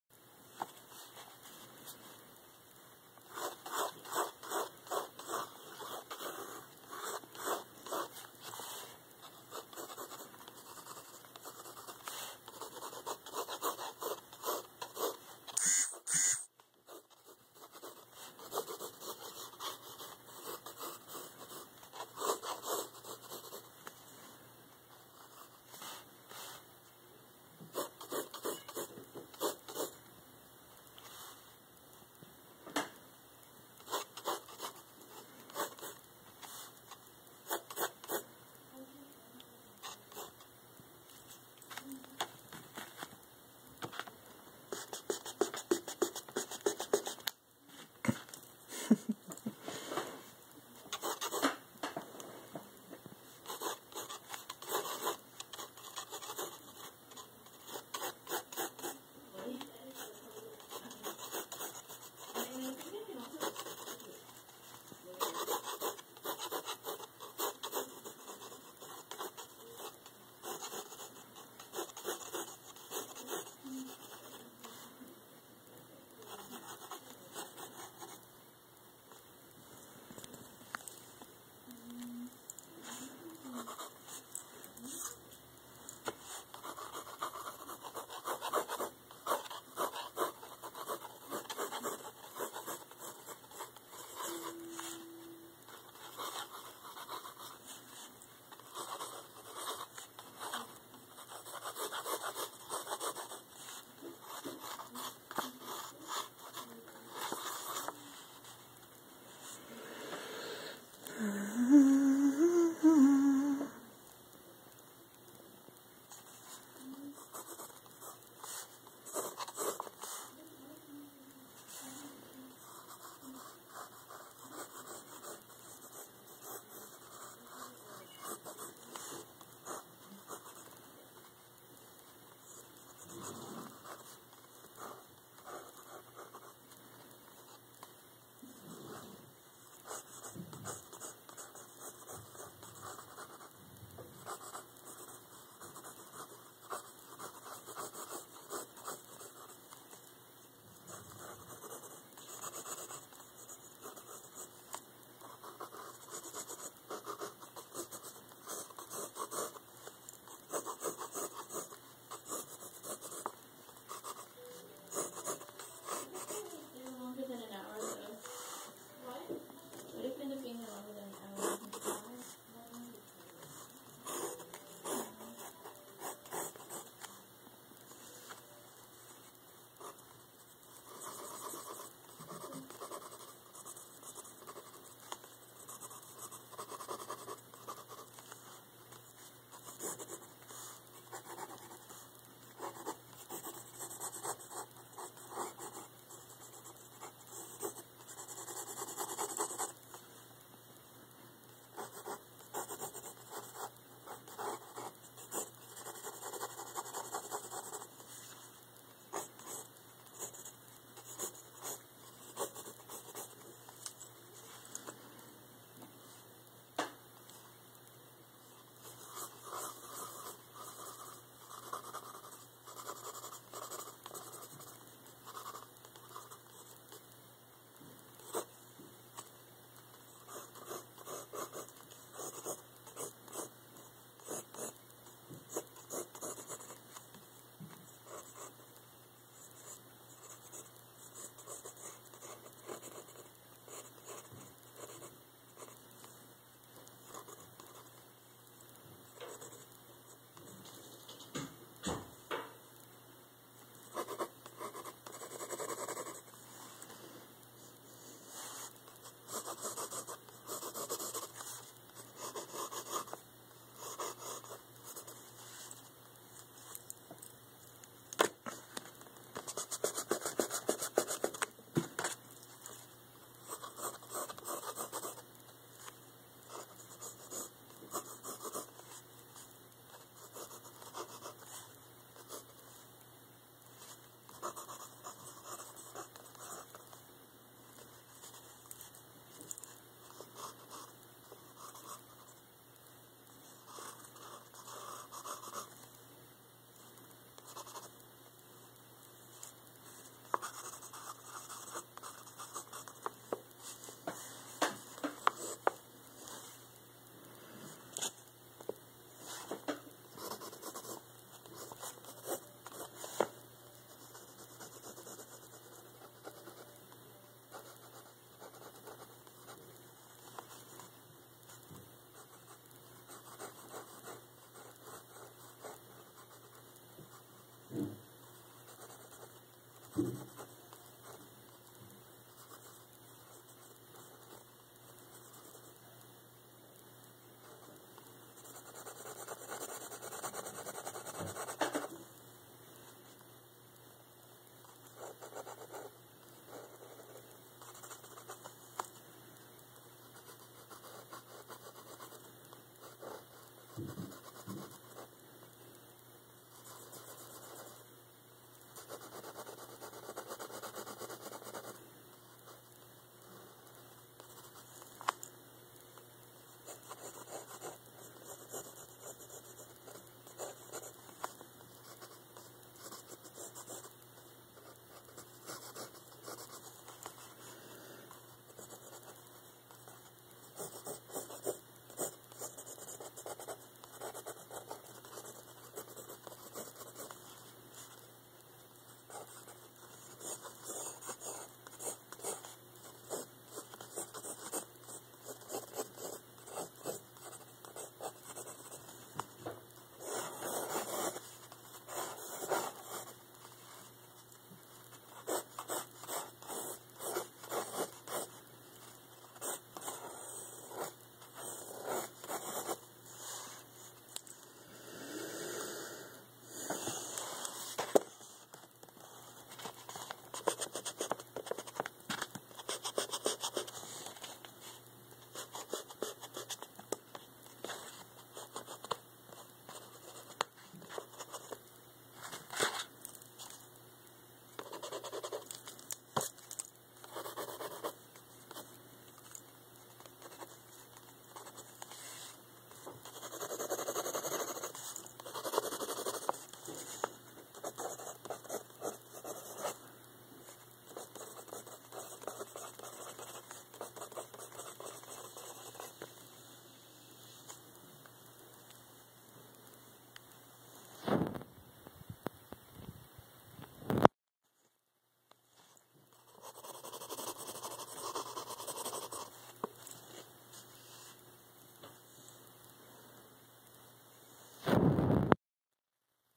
Drawing Byrne
Just a little video of me drawing Byrne from Zelda Spirit Tracks